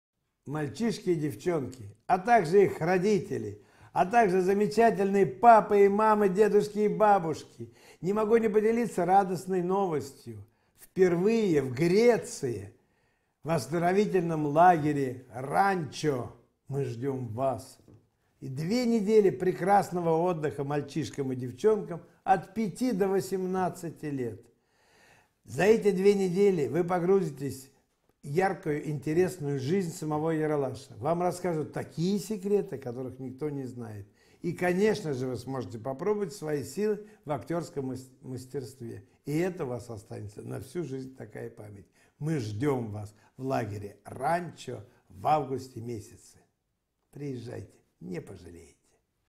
«Ералаш» в Греции I Приглашение от Бориса Юрьевича Грачевского
Приглашение от Бориса Юрьевича  Грачевского для детей от 5 до 18 лет в детский лагерь «Ералаш», который будет проходить в Греции со 2 по 15 августа.